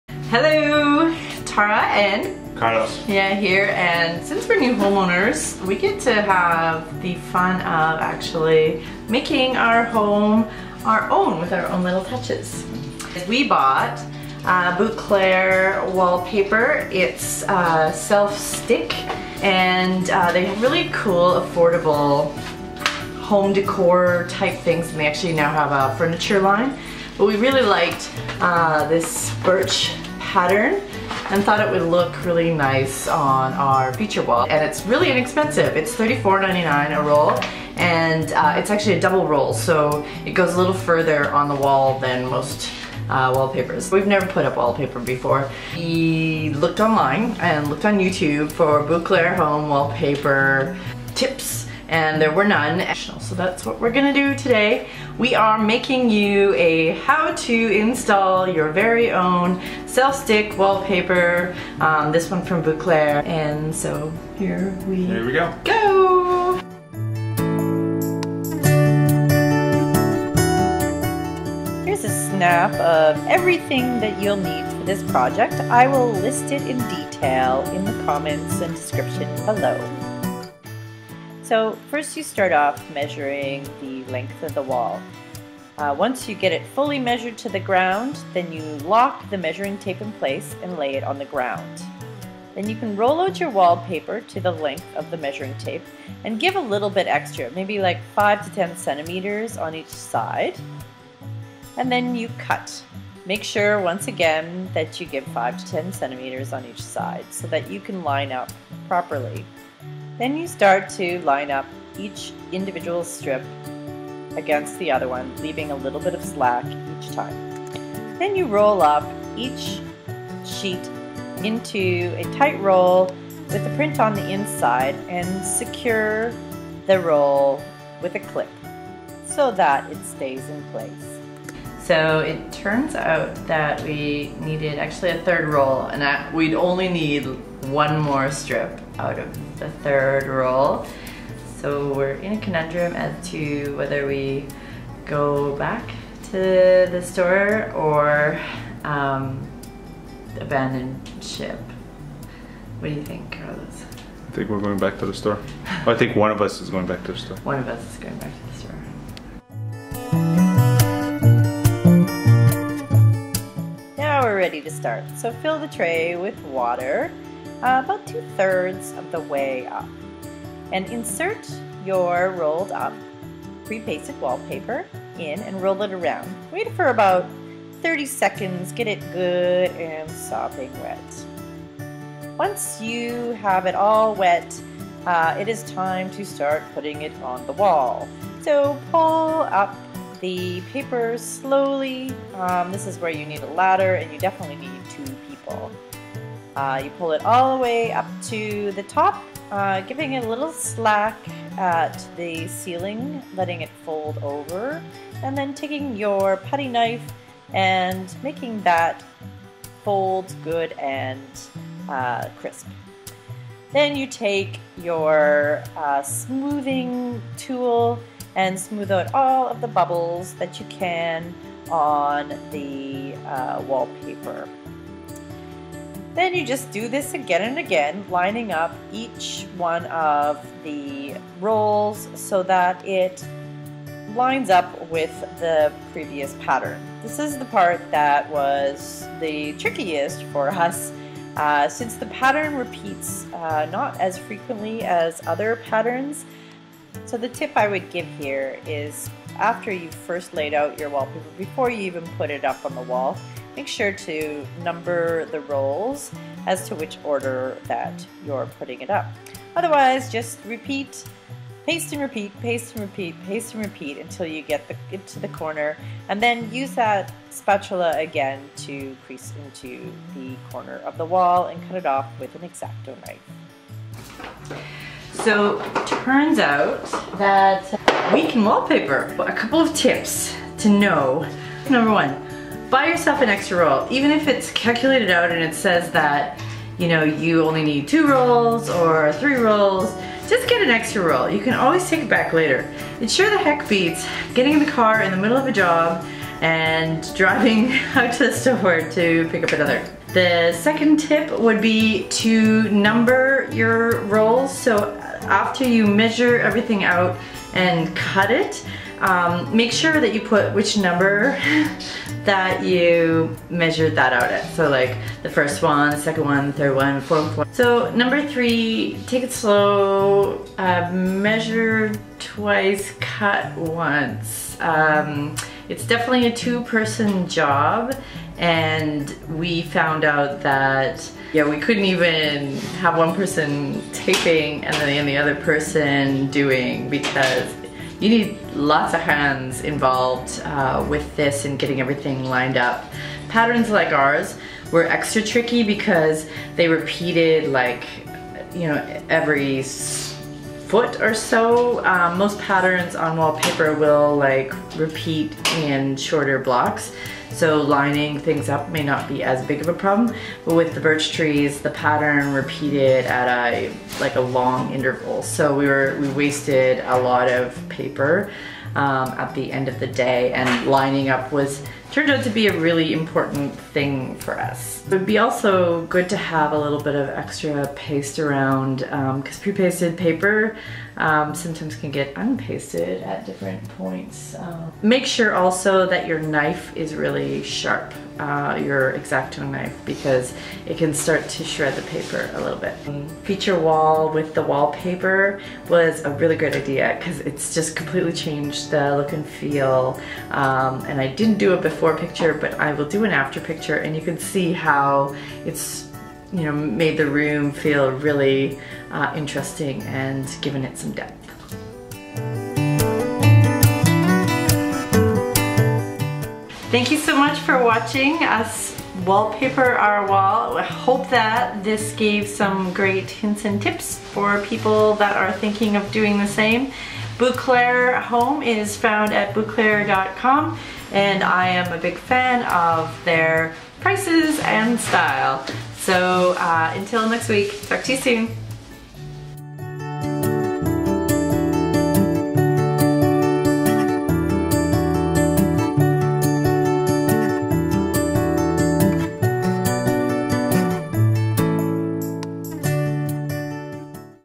How To Hang Pre-pasted Wallpaper for Beginners (paper by Bouclair Home) - DIY
This was our first time putting up wallpaper, but I think we did a pretty great job! What you will need to DIY BouClair pre-pasted wallpaper -

OR
- A wallpaper water tray
- Exacto knife
- Measuring tape
- putty knife
- Wallpaper foam smoother
- Scissors
- Pencil
- Step ladder
- Sponge (optional)
- Sandpaper (if your wall isn't smooth)
- Level (optional)
- Extra wallpaper paste (clear is best)

I quite liked putting up the self-pasted wallpaper. You'll need a bit extra in case for lining up the pattern.

Thanks Carlos for being so awesome and doing this with me even though you felt sick.

Learn about what Truly. does on our